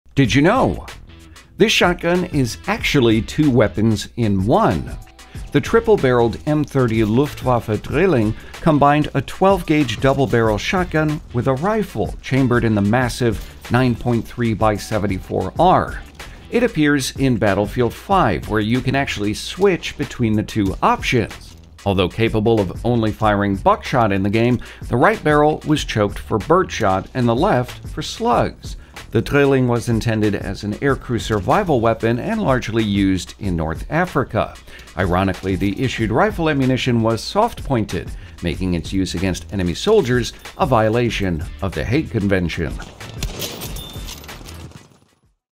This WW2 Shotgun Was 2 Weapons in One
The M30 Luftwaffe Drilling was fine until you flipped that switch. The rounds in-game are FMJ, so it's fine.